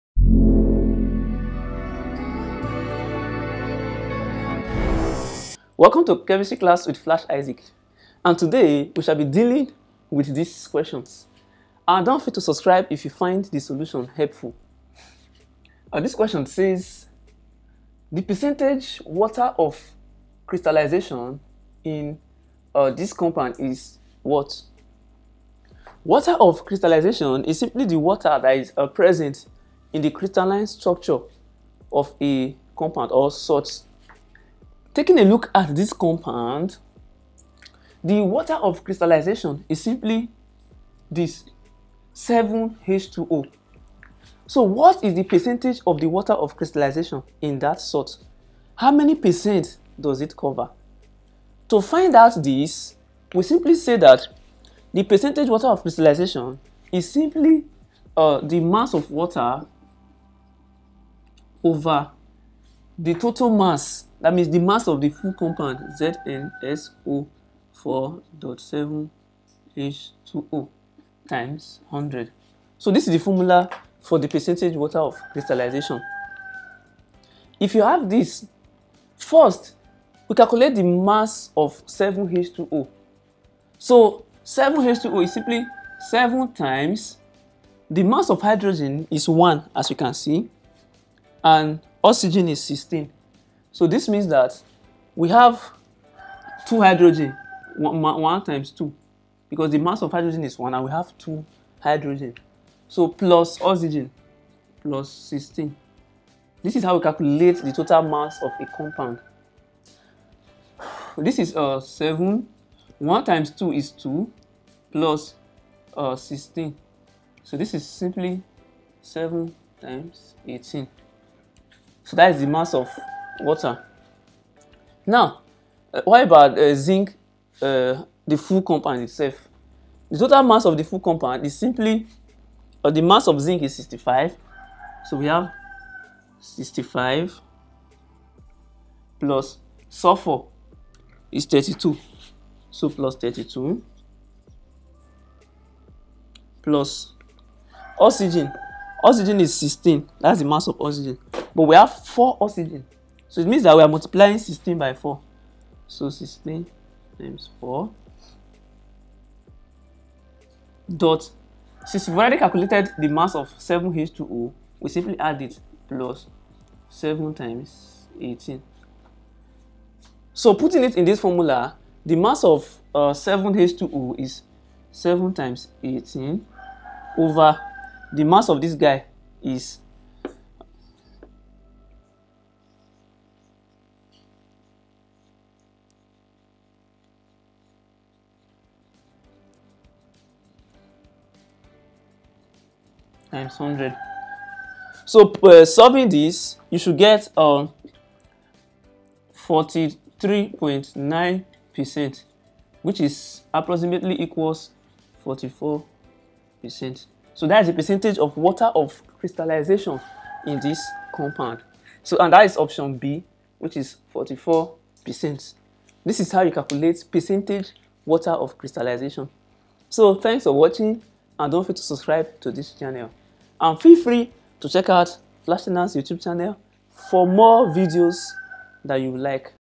% Water of Crystallisation in ZnSO4.7H20
The Percentage Water of Crystallization in ZnSO4.7H20 is? Chemistry Past Question And Answer For Jamb, Waec And Post UTME; Including Likely, Repeated And Related Questions.